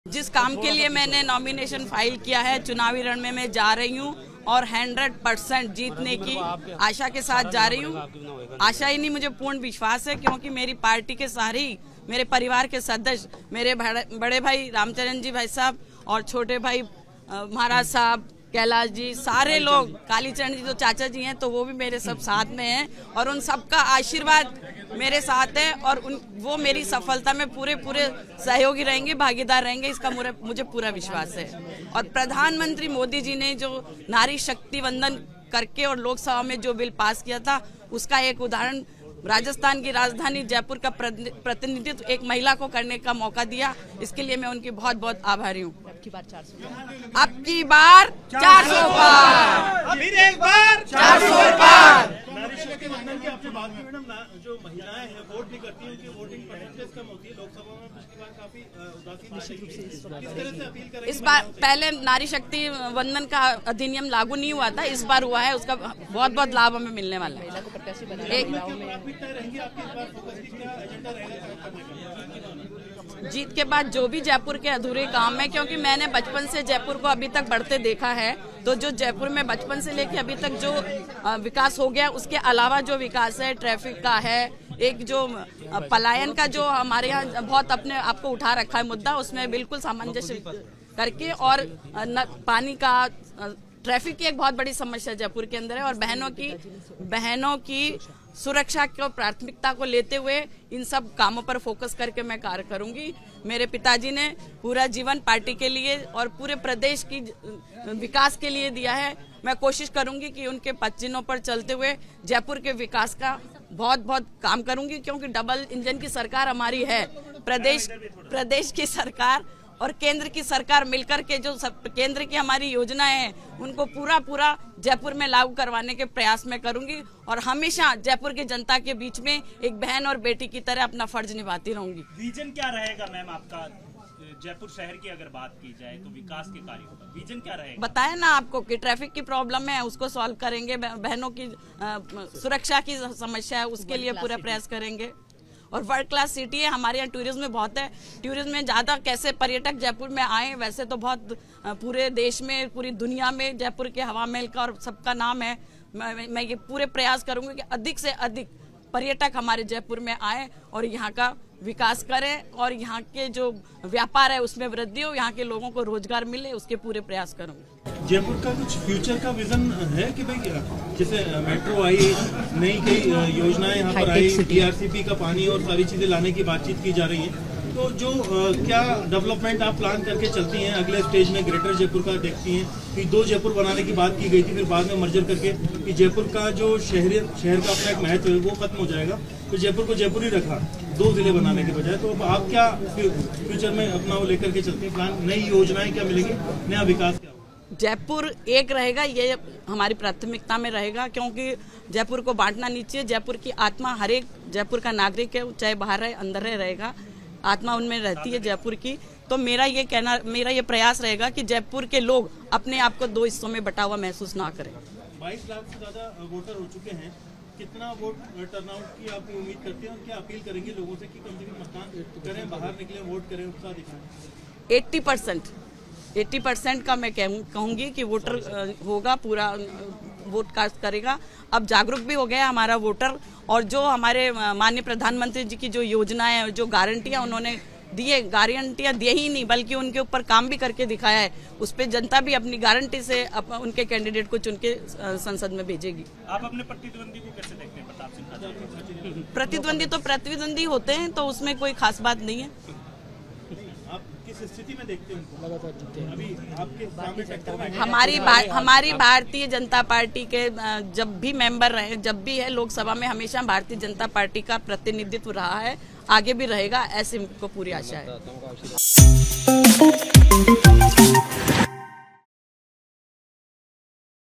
Lok Sabha Election 2024: मंजू शर्मा ने दाखिल किया नामांकन, क्या पुराने रिकॉर्ड पाएंगी? | Jaipur
जयपुर शहर की लोकसभा सीट के लिए मतदान 19 अप्रैल को होना है और दोनों प्रमुख दलों बी जे पी और कांग्रेस के उम्मीदवार तय होने के बाद अब चुनाव प्रचार गति पकड़ता दिख रहा है। बी जे पी में इस बार महिला ब्राह्मण चेहरे मंजू शर्मा को अपना प्रत्याशी बनाया है तो कांग्रेस ने पूर्व मंत्री प्रताप सिंह खाचरियावास को.